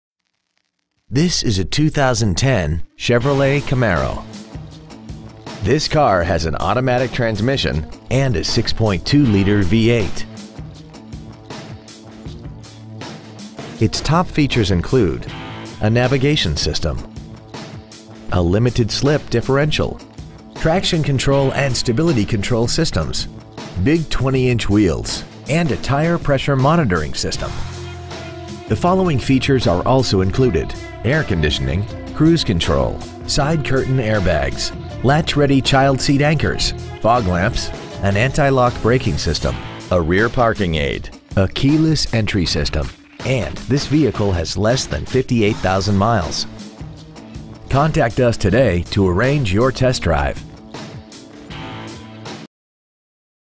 2010 Chevrolet Camaro Irving TX
Year : 2010
 Make : Chevrolet
 Model : Camaro
 Engine : 6.2L V8 SFI Engine
 Trans . : Automatic
 Exterior : Gray
 Miles : 57,355

 2010 Chevrolet Camaro Dallas TX